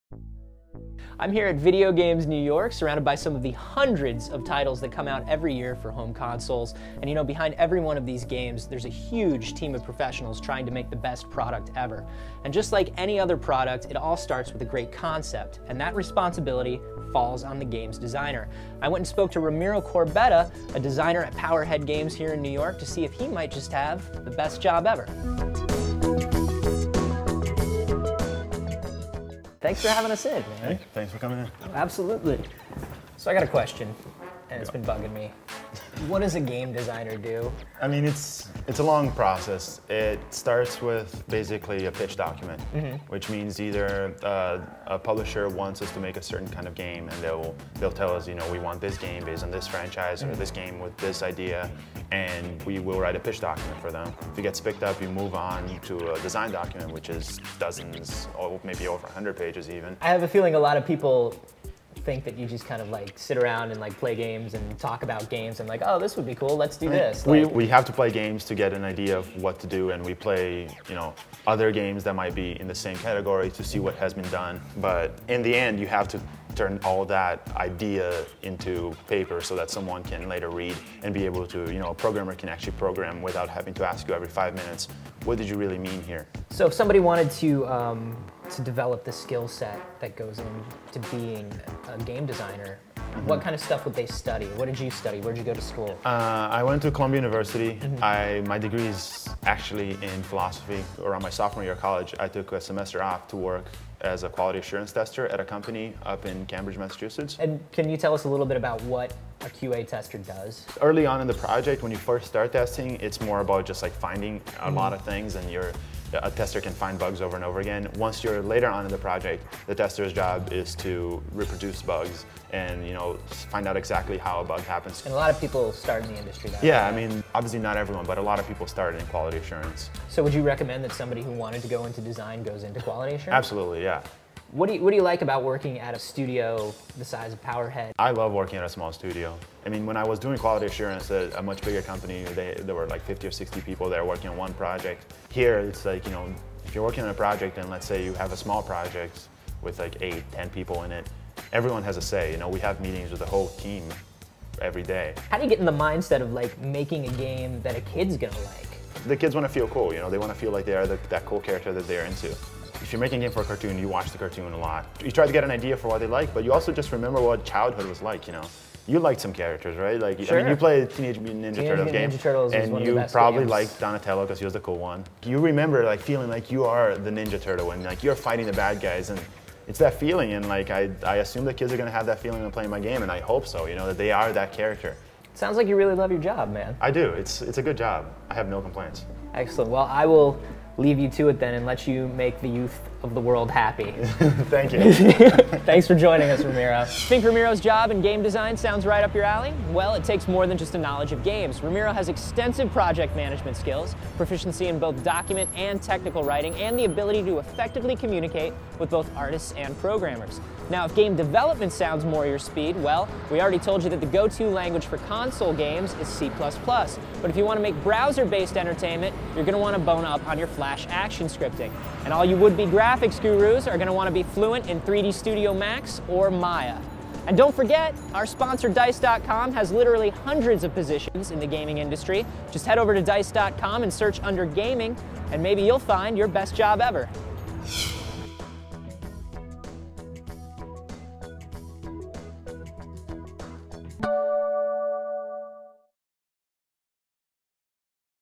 Meet Ramiro Corbetta, Game Designer at Powerhead Games (2009)
Ramiro has turned his philosophy degree and quality assurance experience into one of the best jobs ever: Game Designer. While playing video games is a key part of Ramiro's job (you gotta know your product!) the workday of this game designer is spent driving the design process from pitch document to finished profit-making product - and doing intense industry research.